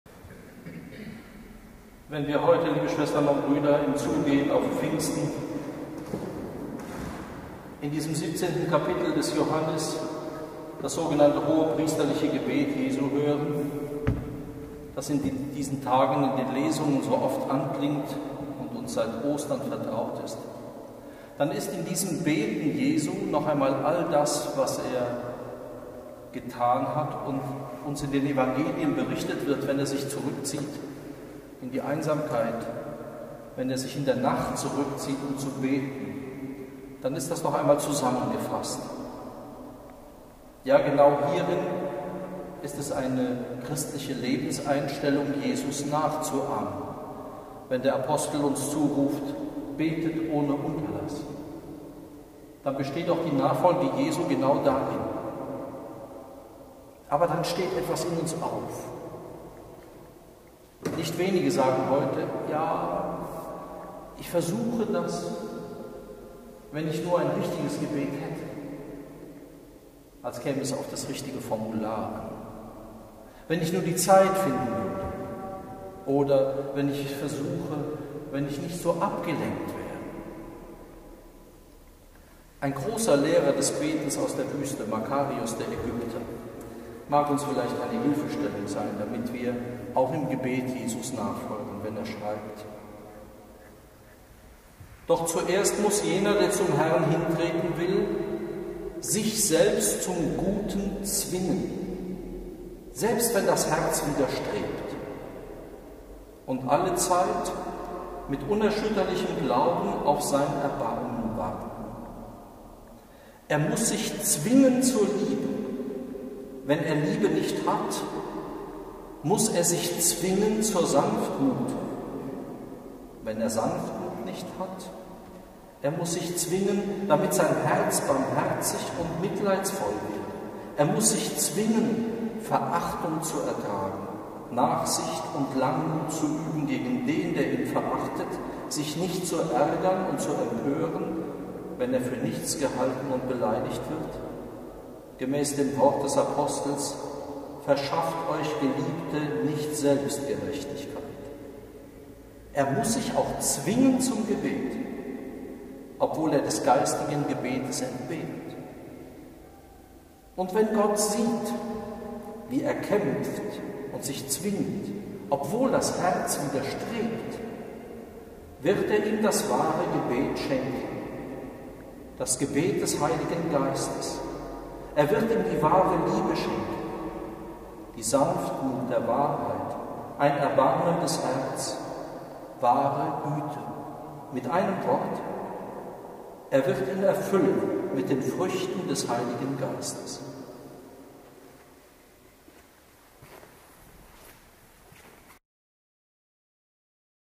Von den Früchten des Hl.Geistes erfüllt werden
Predigt von Rektor Dr.Johannes J.Kreier am Di der 7. Woche nach Ostern 2018 im Kloster am Rande der Stadt/ SB .